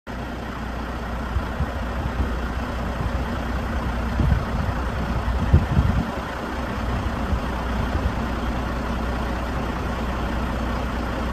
2004 KENWORTH T800 For Sale
2004 KENWORTH T800 TANDEM AXLE, AIR RIDE SUSPENSION DAYCAB TRUCK WITH CATERPILLAR C15 TURBO DIESEL ENGINE AND EATON FULLER 10 SPEED MANUAL TRANSMISSION. ODOMETER SHOWING 185,442 MILES. ECM SHOWING 834,741 MILES AND 30,091 ENGINE HOURS! MUST BE SOLD TRUE MILES UNKNOWN! VERY HARD TO FIND, GREAT RUNNING TRUCK!